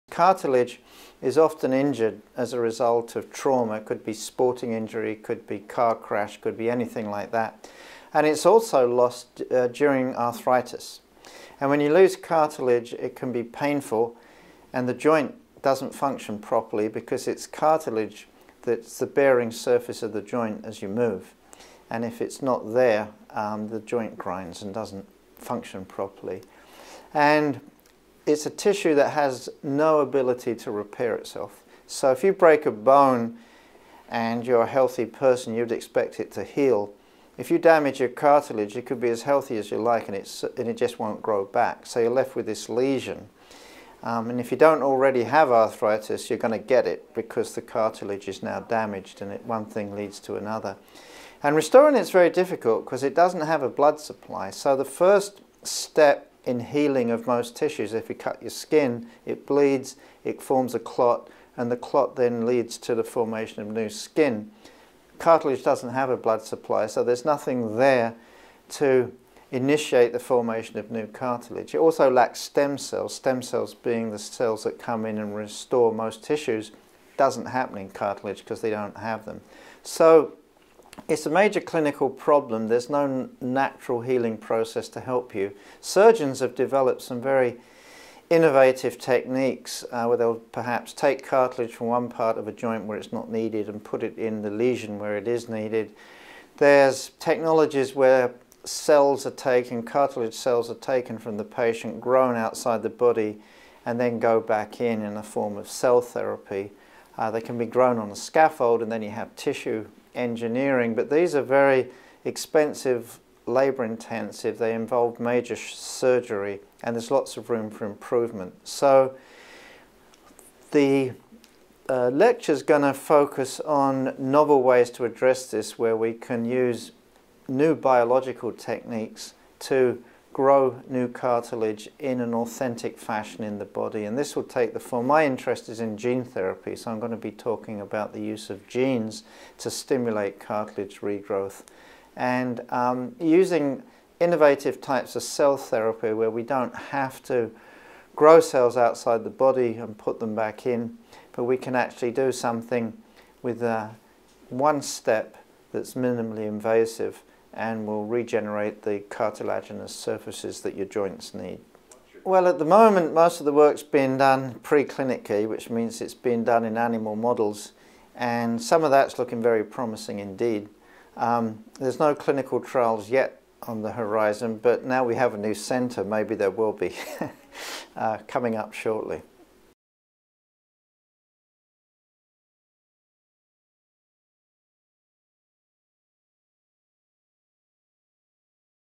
Cartilage Regeneration
Christopher H. Evans, Ph.D., Director of Rehabilitation Medicine Research Center, Mayo Clinic PM&R, discusses his lecture at AAPM&R 2014 on the damage done to cartilage from trauma and arthritis. He focuses on the difficulties of healing and regenerating cartilage. His lecture plans to touch on novel ways use new biological techniques to grow new cartilage, including gene therapy.